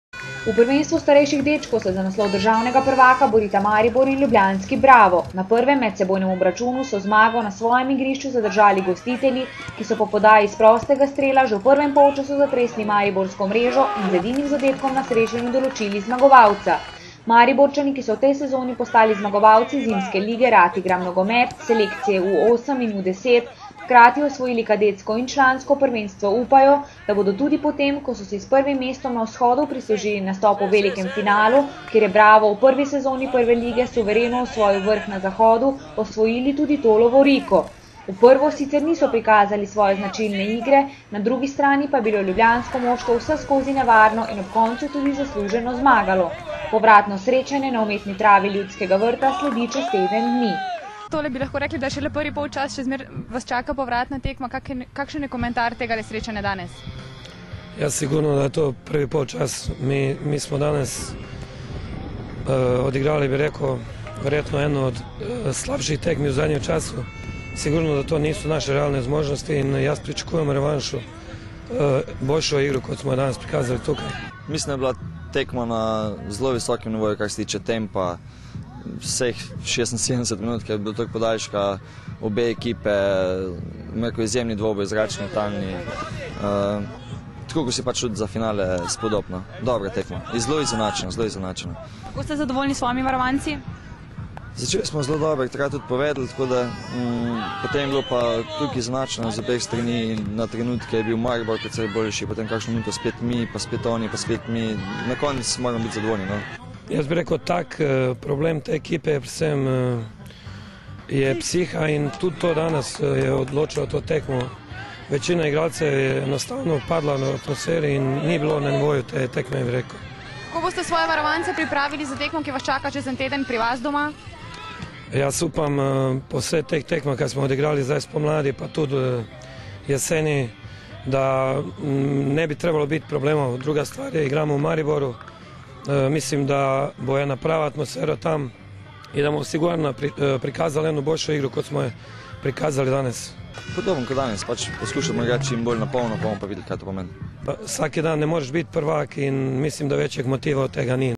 Finale U-14 Bravo Publikum : Maribor
Prva tekma finala starejših dečkov Bravo Publikum : Maribor 1:0 (1:0)